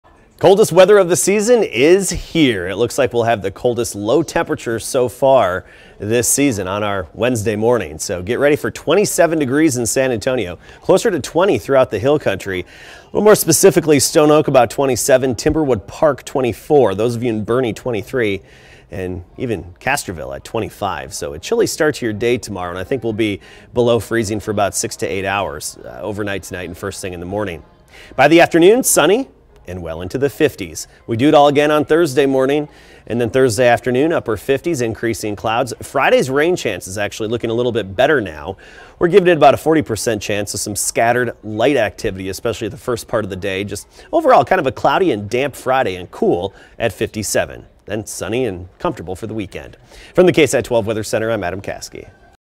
Adam's 10pm Update: Tuesday 12/17/19
Coldest temperatures so far this season are upon us..